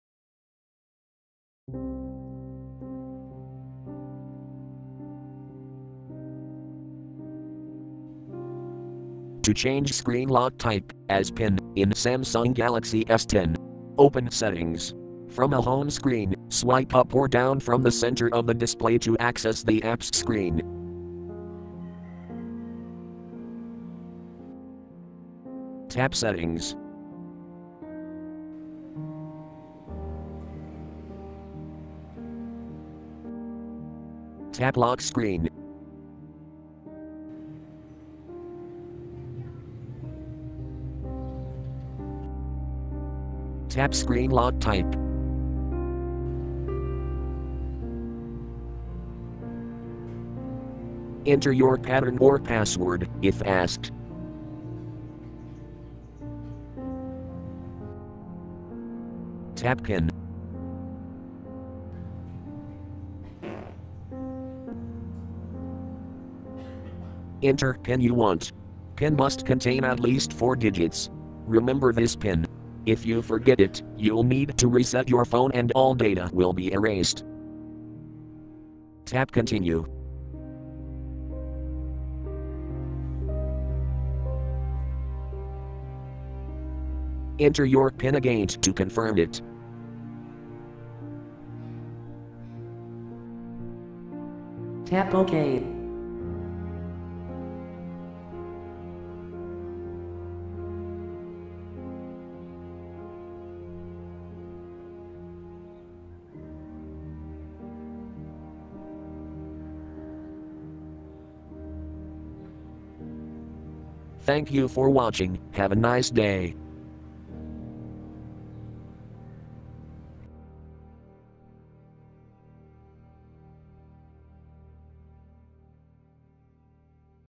Samsung Galaxy S10 : How to change screen lock type as PIN
This video show How to change screen lock type as PIN in Samsung Galaxy S10.  In this tutorial I use Samsung Galaxy S10 SM-G973F/DS Duos International version with Android 9.0 (Pie).